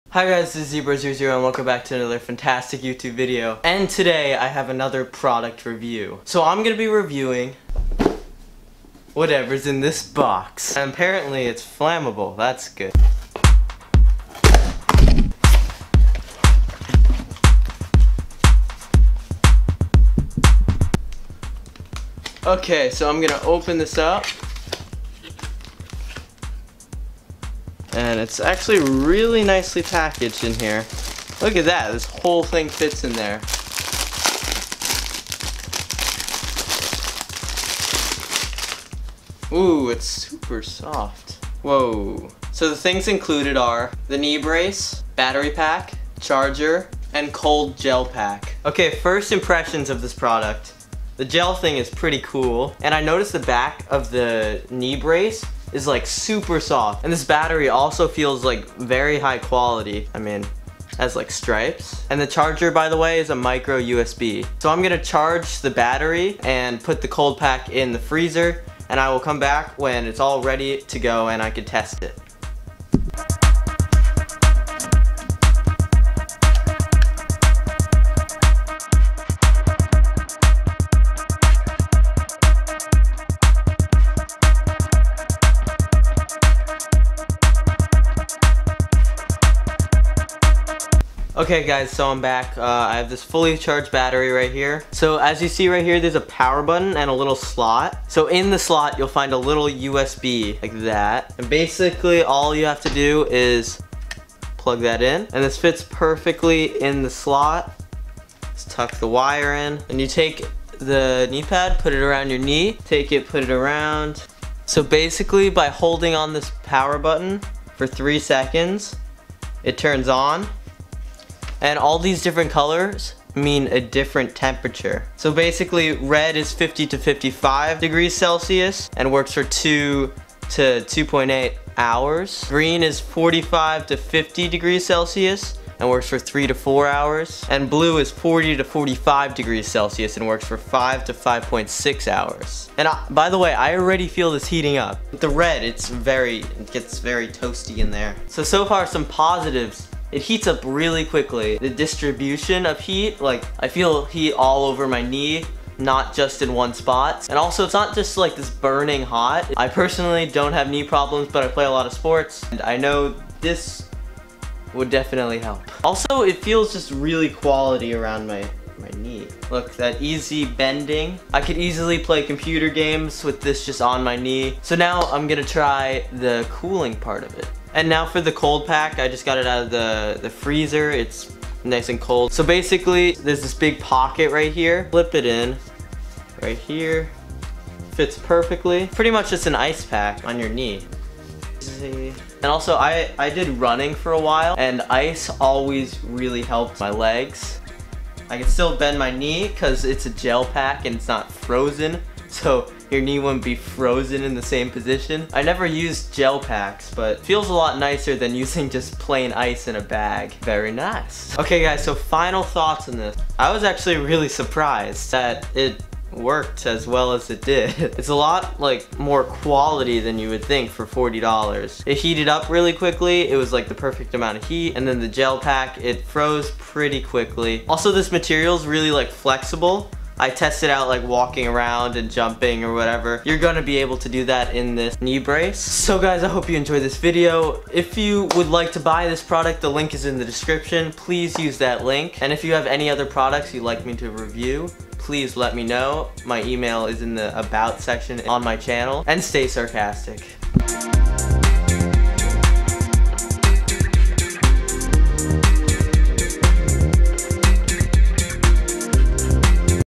Fitfirst Heating Knee Pad (FULL REVIEW)
Hi guys this is Zebros00 and today I'm reviewing the Fitfirst Knee Heating pad, 2 in 1 USB Powered Heat/Cold Therapy Knee Brace Wrap with 3 Heat Settings for Joint Pain, Arthritis Pain Relief, Ice Gel Pack and 5200mAh Power Bank Included. I hope you enjoy!!!

ZEBROS00 SHOP